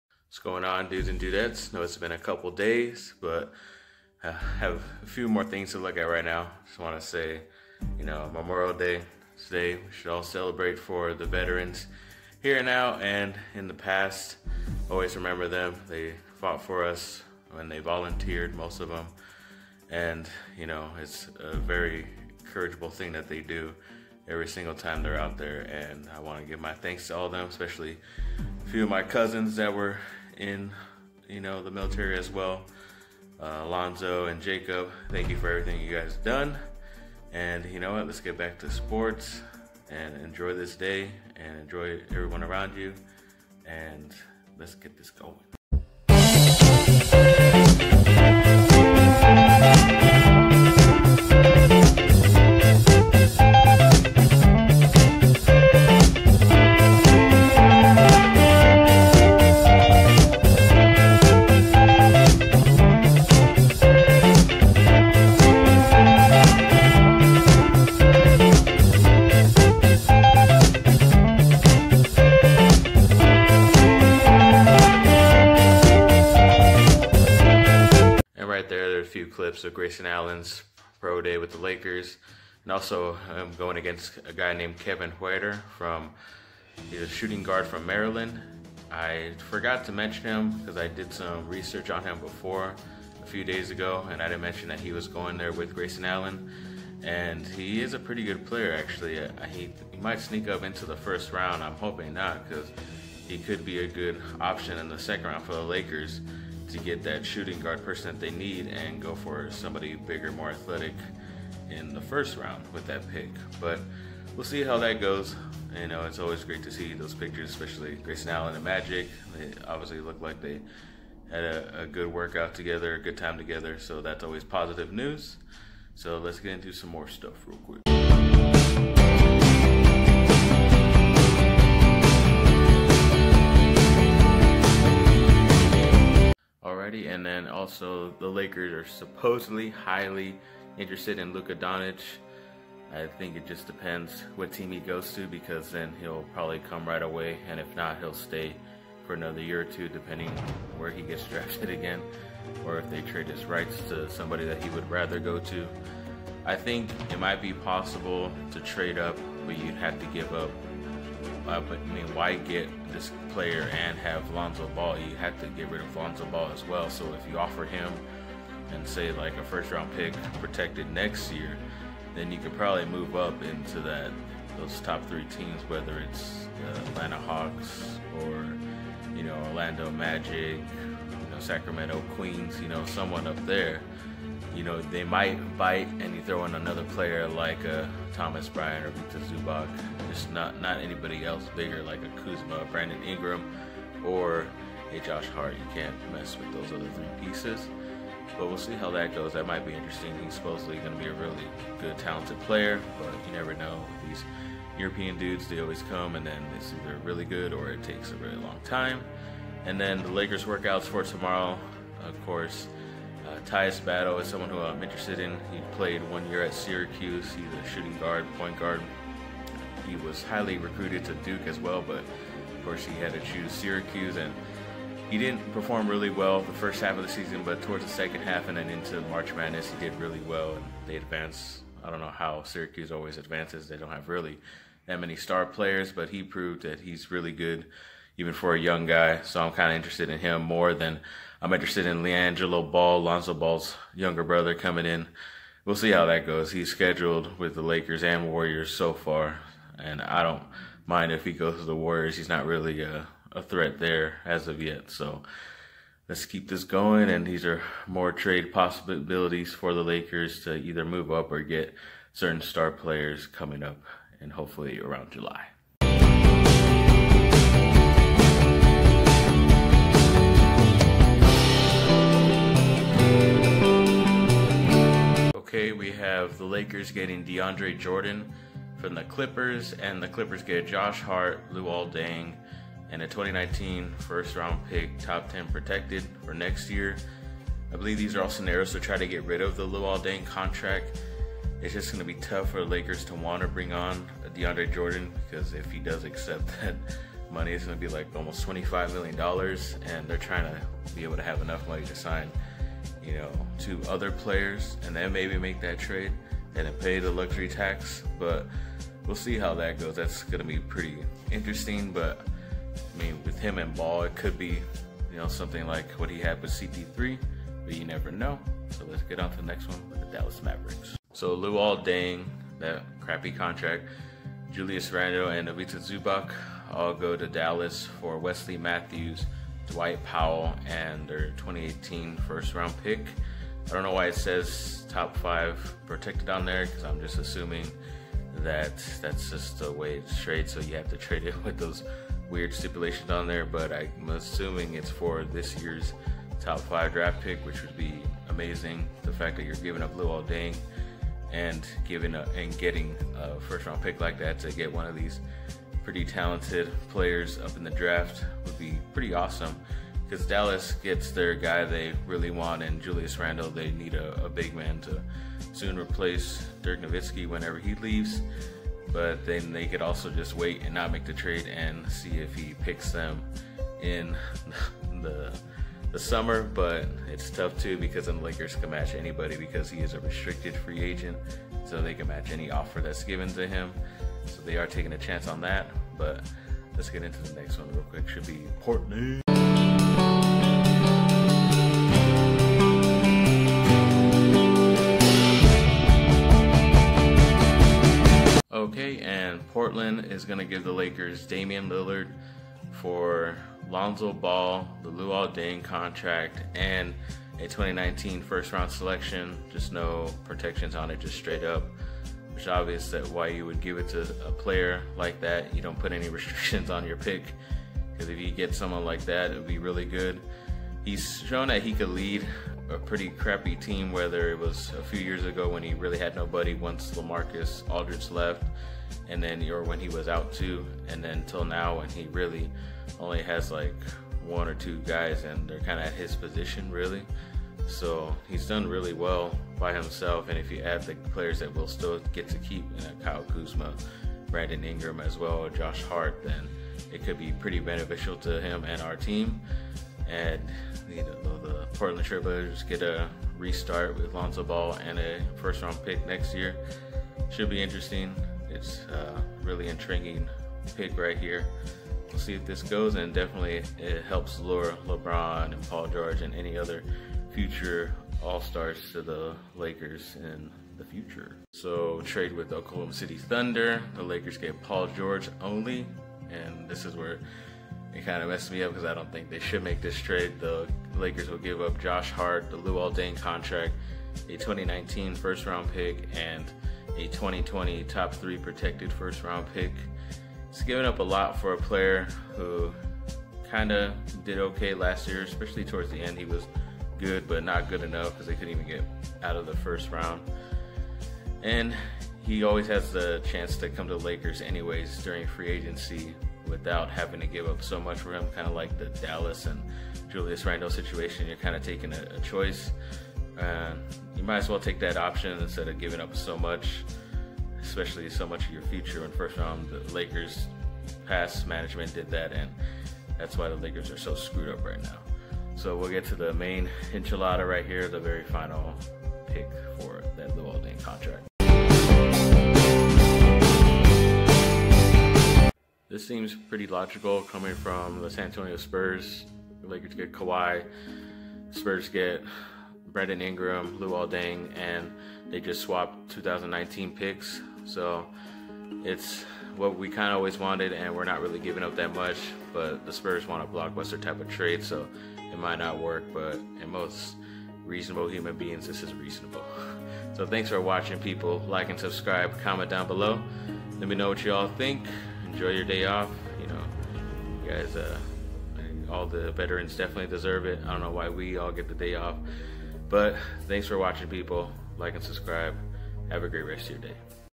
Lakers News: Lakers Trade For All Star? Allen & Huerter Draft Options For Lakers! Doncic To Lakers??
Grayson Allen and Kevin Huerter Draft Options For Lakers

Lakers High On Doncic, Battle and Ball Workout For Lakers Tuesday

Lakers Trade For All Star, DeAndre Jordan, Damian Lillard, Paul George, Kawhi Leonard